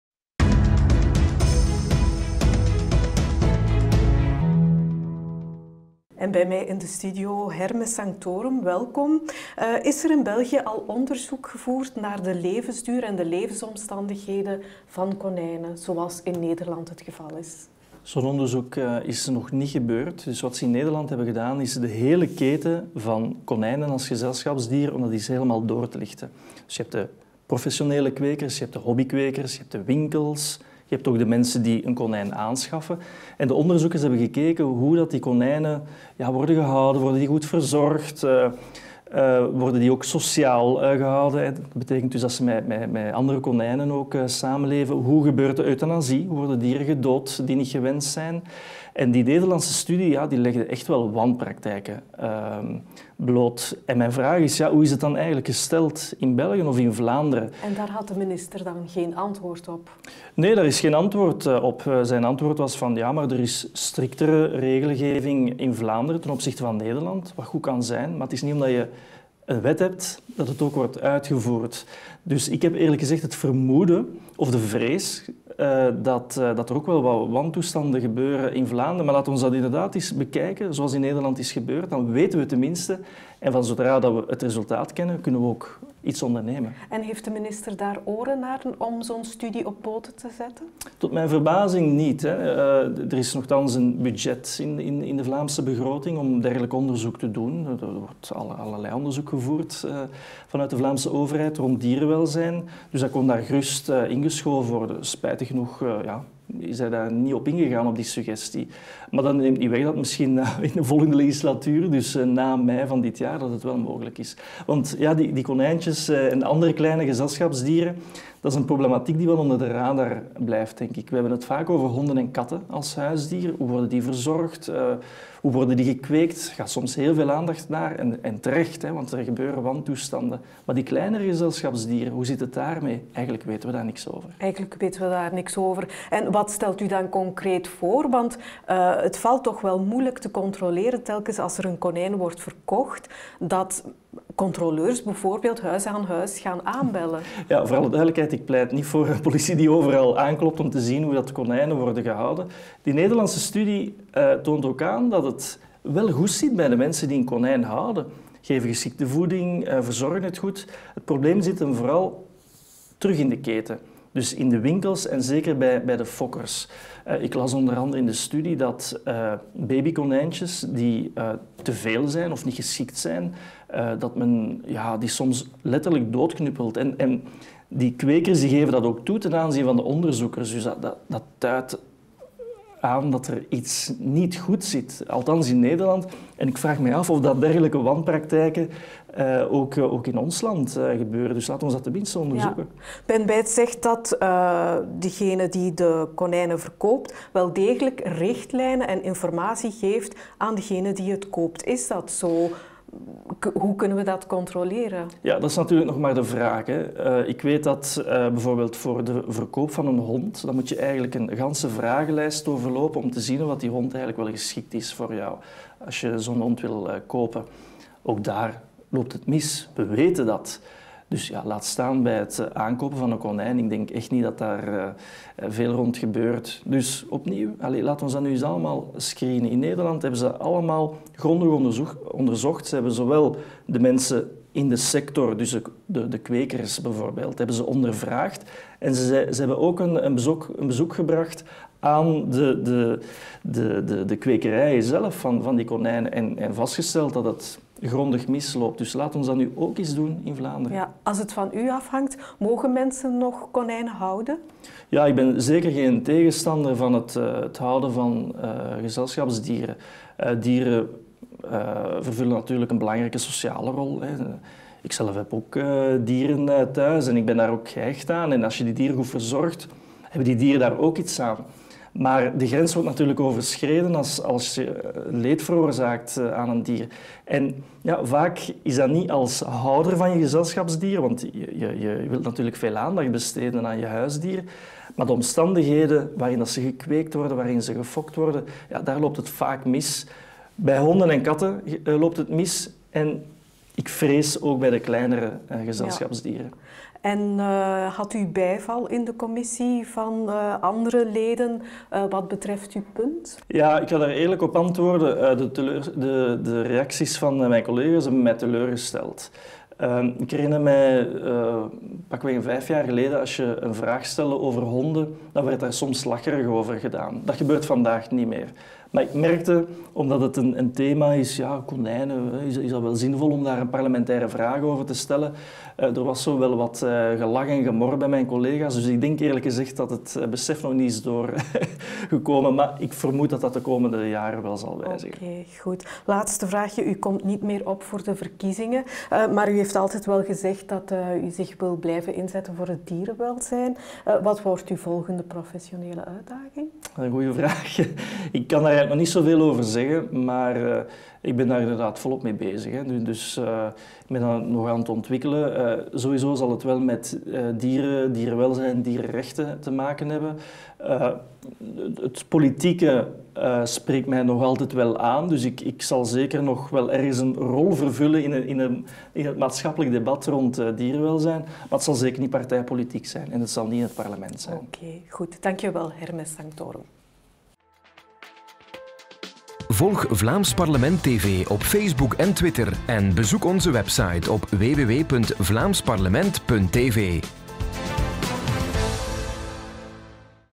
Studio Commissie: Hermes Sanctorum over de kweek en verkoop van konijnen
Uit een studie van de Universiteit Utrecht blijkt dat het niet goed gesteld is met het huiskonijn. Hoewel de dieren meer dan 10 jaar oud kunnen worden, sterven ze gemiddeld reeds op de leeftijd van 4,5 jaar. De oorzaak daarvoor zou te wijten zijn aan slechte omstandigheden in alle schakels van de keten.

Maar volgens minister van Dierenwelzijn Ben Weyts (N-VA) is de regelgeving in Vlaanderen veel omvattender dan in Nederland. “Je kan ze niet met elkaar vergelijken. In Vlaanderen geldt er een duidelijkere bescherming”, reageert de minister. Hermes Sanctorum (Onafhankelijk) vraagt zich af of deze wetgeving effectief wordt opgevolgd.